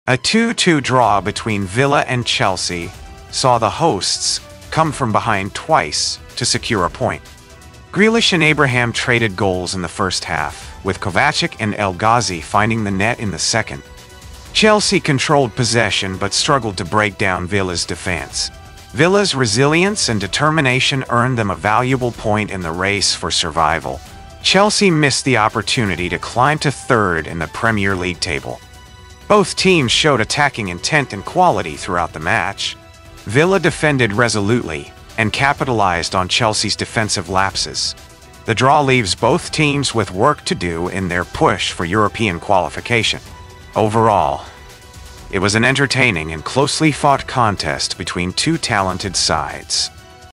Aston Villa 2-2 Chelsea: Disasi's late winner controversially denied as Blues rally
a gripping encounter between Aston Villa and Chelsea, the match unfolded with intensity and drama, culminating in a 2-2 draw that left fans on the edge of their seats. Axel Disasi's late goal appeared to seal a dramatic victory for Aston Villa, only to be met with controversy as it was disallowed, denying them the win. The Blues, trailing behind, staged a remarkable comeback to level the score, adding to the tension of the match. Both teams showcased skill and determination, captivating audiences with their thrilling performance on the field. Despite the disallowed goal sparking debate, the match will be remembered for its exhilarating moments and the competitive spirit displayed by both sides.